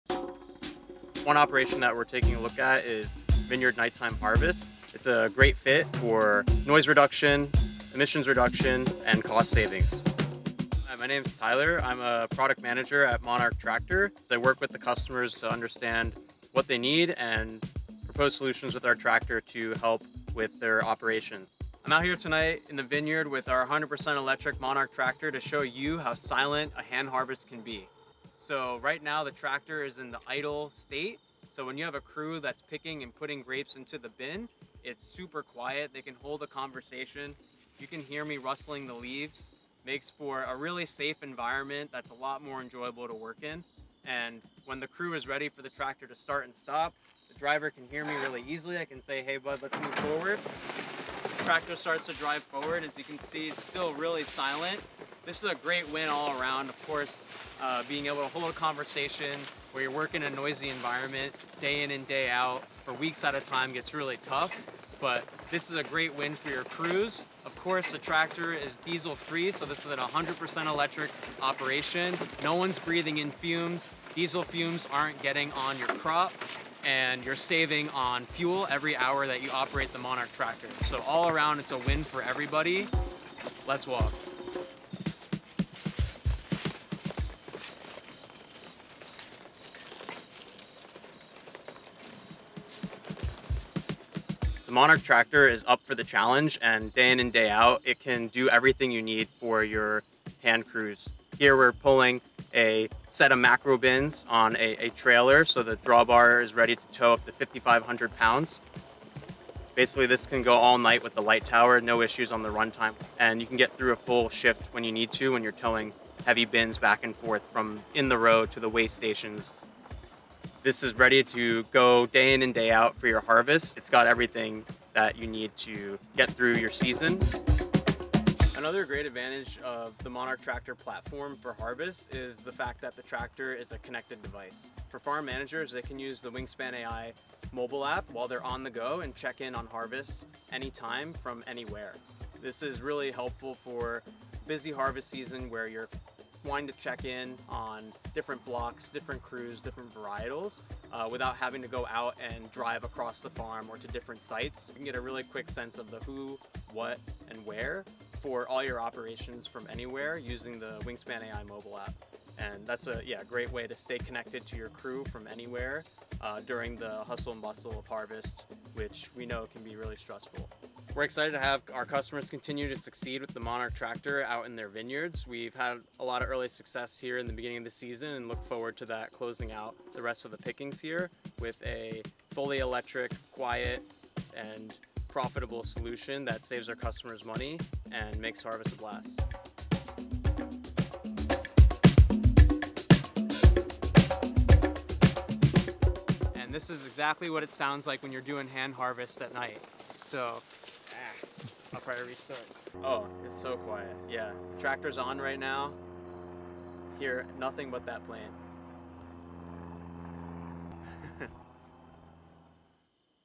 Electric Tractor for vineyard night harvest | Monarch Tractor
Preparing for Harvest with a Monarch Tractor. Bright lights, quiet operations, and no diesel fumes...

00:00 Introduction
0:23 Walking in the rows
1:34 Hauling harvest bins
2:07 Using the WingspanAI App during harvest
2:51 Wrap up!

Whether you're just starting or already in full swing, your MK-V is here to make your operations quiet, diesel-free, and more efficient by hauling bin trailers or powering lights.

Going electric with your MK-V means a more affordable harvest, cutting diesel costs, and providing a safer environment for your operators—all while delivering better quality grapes.